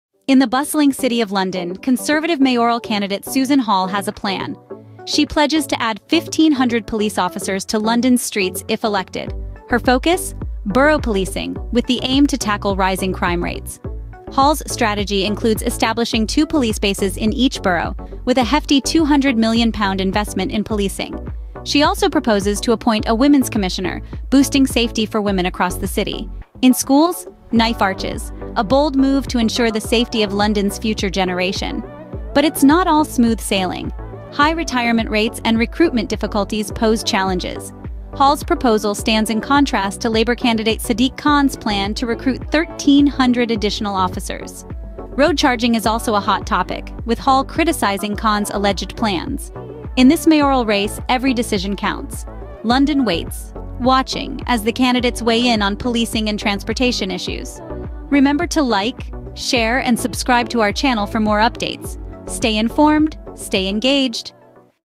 London mayor elections: Susan Hall promises 1,500 extra police
The Conservative candidate for the London mayoral race has made a bold pledge to add an "extra 1,500 police officers" to the streets of the capital if elected. Susan Hall emphasized her commitment to reinstating borough policing as a means to combat crime, proposing the establishment of two police bases in each borough as part of a £200 million investment in law enforcement.

Hall's announcement comes in response to her opponent, Labour candidate Sadiq Khan, who had previously stated his intention to recruit 1,300 additional officers if he wins the election. Highlighting her concerns over rising crime rates, Hall stressed the importance of having a visible police presence in communities.

Speaking on her policing policies, Hall expressed her determination to address the escalating crime situation, citing the need for police resources on the ground. She outlined plans to set up two bases in each of the 32 boroughs, clarifying that while these bases would primarily serve as hubs for police operations, not all would offer public-facing services.

Funding for these bases would be sourced from a combination of broader policing budgets and potentially repurposing existing facilities in some boroughs. Additionally, Hall proposed the appointment of a women's commissioner dedicated to enhancing safety for women in London.

In an effort to tackle knife crime, Hall announced plans to install knife arches in every London school, aiming to create safer environments for students. She also emphasized the importance of recruiting more officers into the Metropolitan Police, highlighting previous recruitment challenges faced by the force.

Meanwhile, the campaign also touched upon the issue of road charging, with Hall reiterating claims that Khan intends to introduce "pay-per-mile" charges, a notion vehemently denied by Khan's spokesperson, who labeled it as Tory misinformation and scaremongering.

As the race for the London mayoralty heats up, Hall's proposals signal a clear focus on public safety and law enforcement, contrasting with the rebuttals and assurances from her opponents.